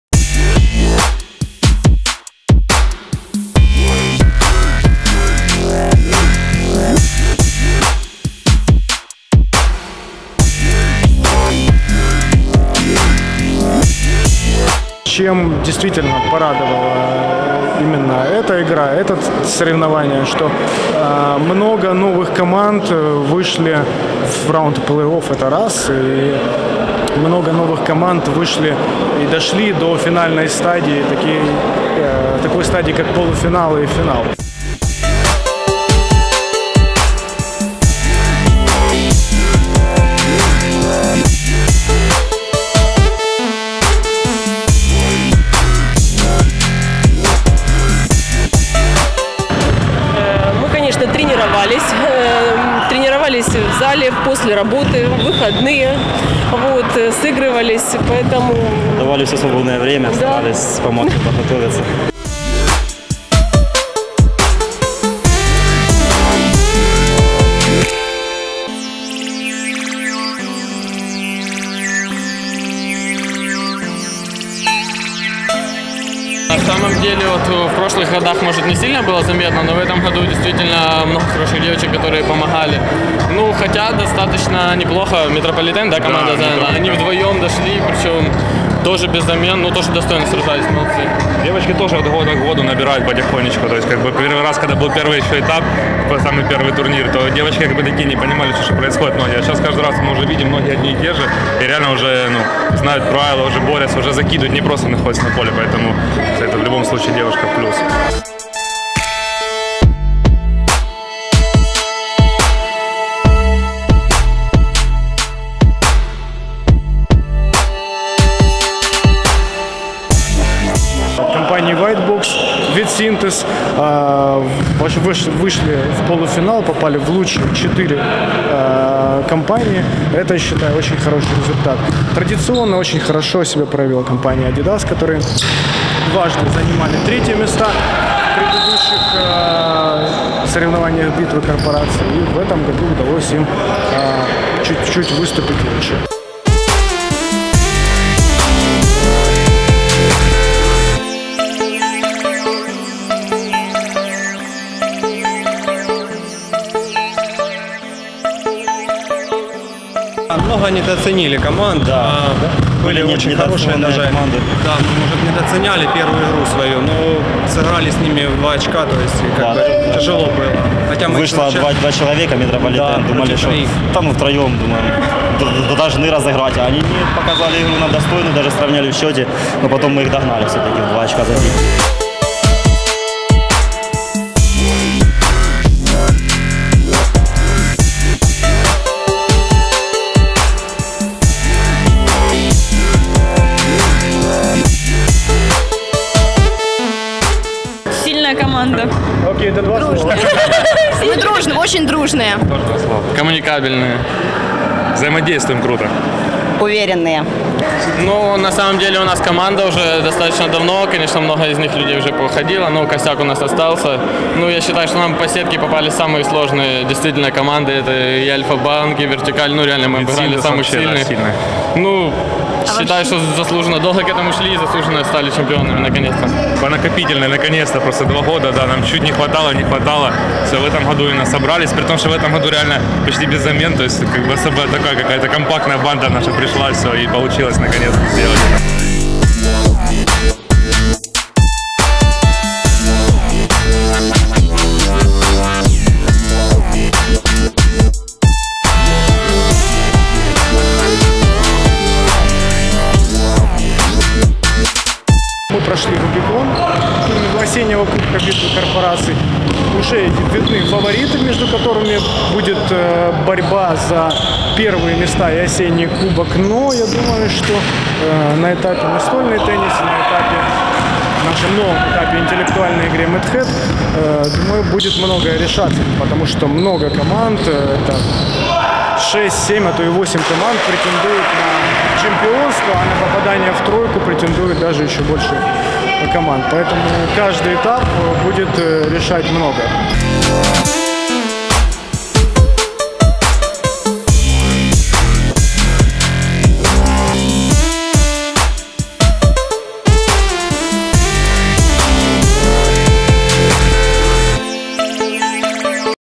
Битва корпораций, сезон 2018/2019 (IІI этап - Стритбол)
«Битва корпораций» - спортивный корпоративный турнир, в котором принимают участие спортсмены-любители, представляющие свою компанию. Соревнования проходят в течении 10 месяцев (с сентября по июнь).